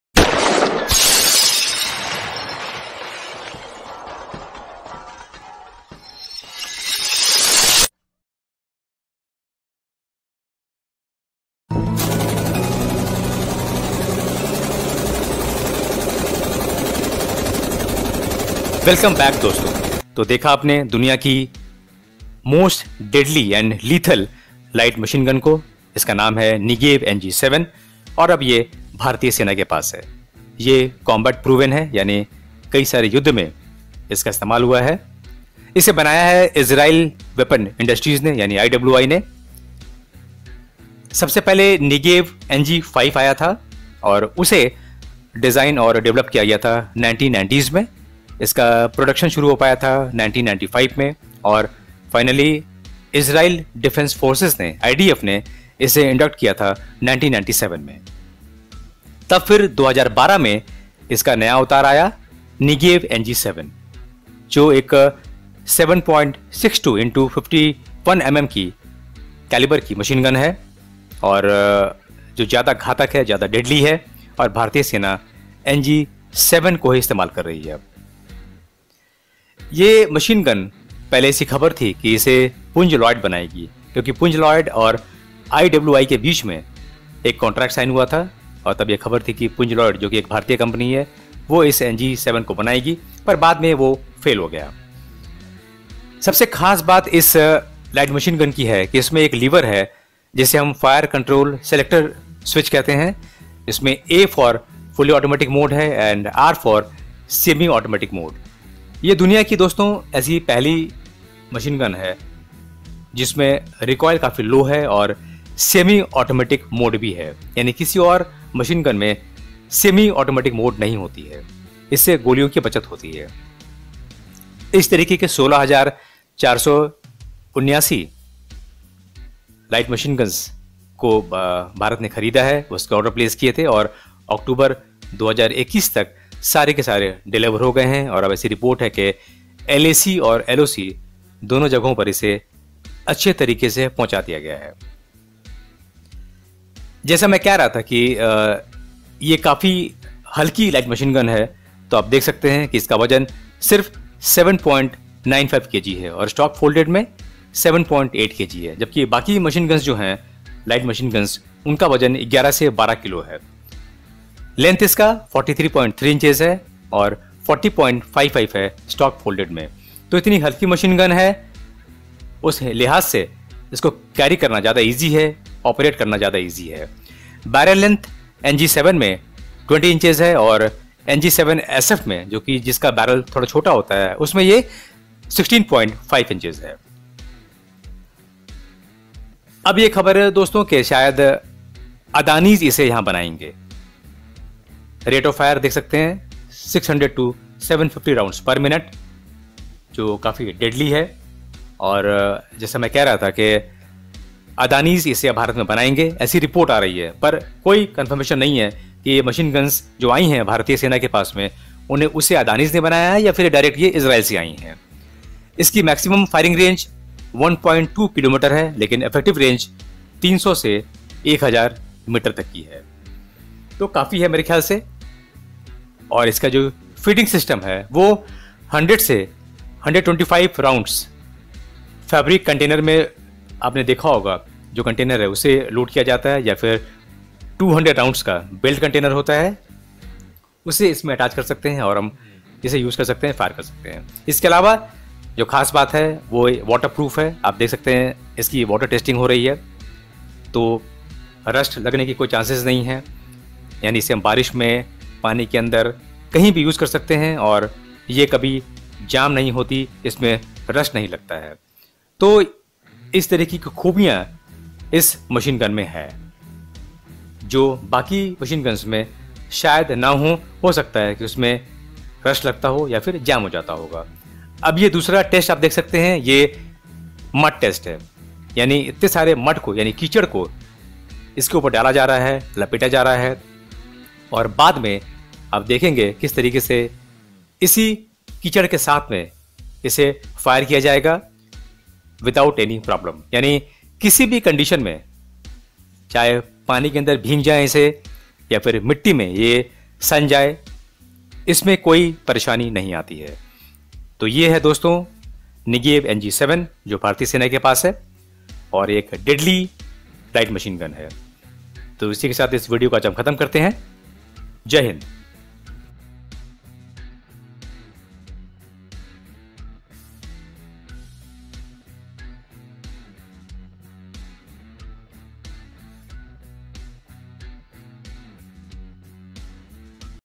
Indian Army Negev LMG l Negev NG7
Indian Army Negev LMG. Negev NG7

Highlights:
What is the Negev LMG?
How Good Is Negev LMG?
Is Negev LMG Best in the world?
Negev NG7 LMG